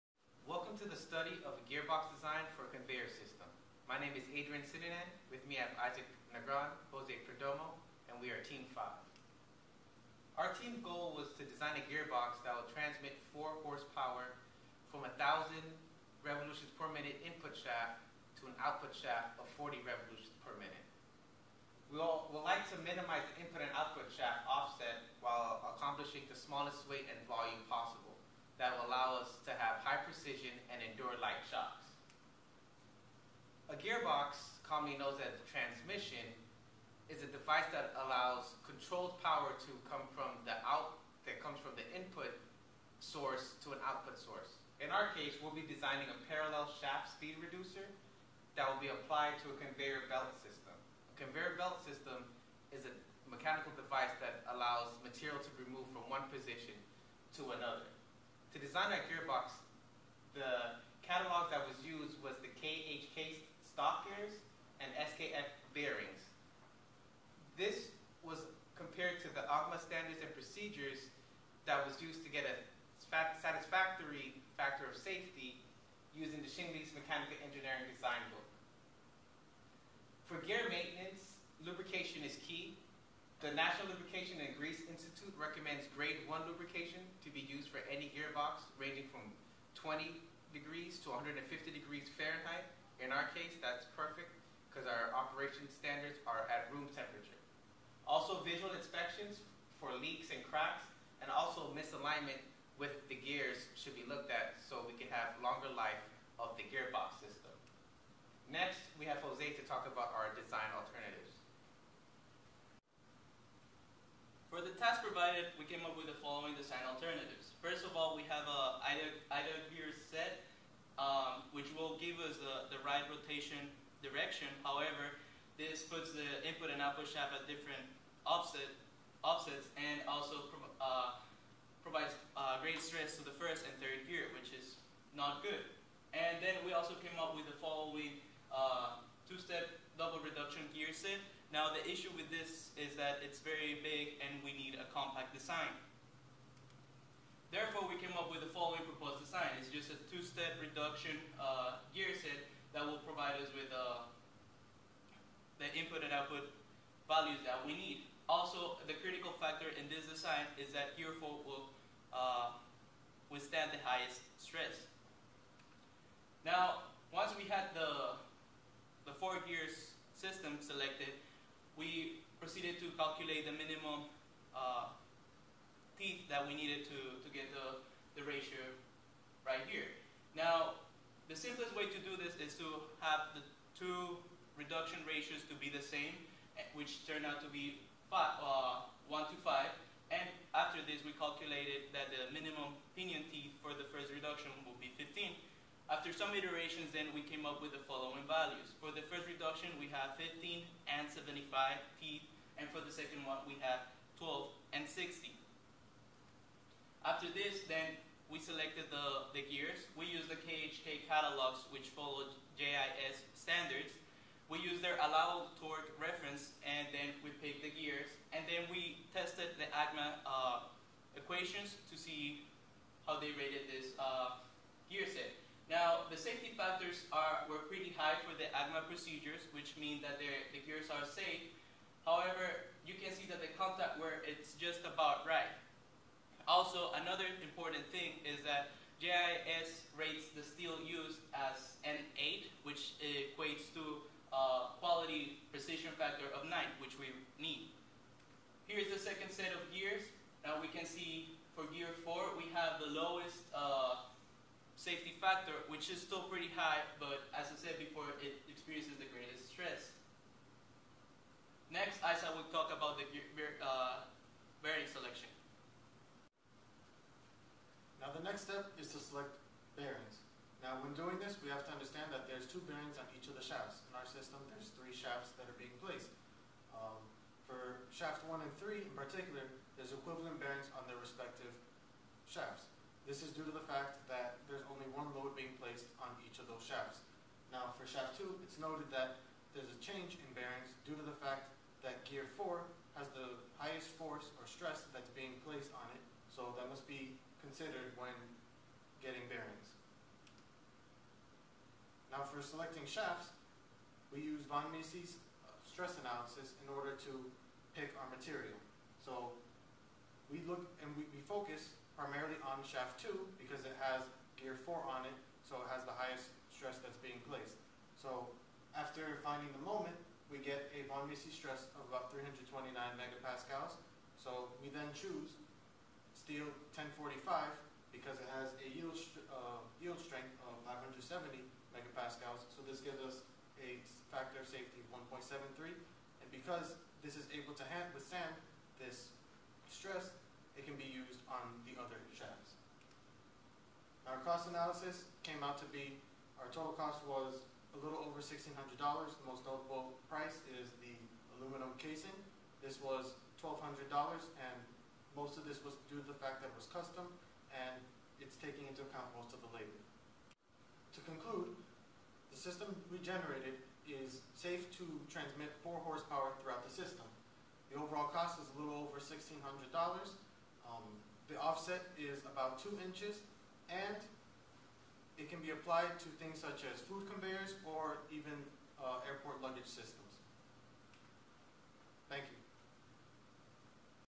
Team 5: Gearbox Design for Conveyor Systems
Izak Negron, Jose Perdomo, Adrian Sinanan

Design of a gearbox for possible use in a conveyor system. This project was developed as part of the course requirements in Mechanical Design II course.

Department of Mechanical and Materials Engineering
Florida International University
10555 West Flagler Street

Professor Dr. Sabri Tosunoglu
TA: Neha Chawla
Fall 2016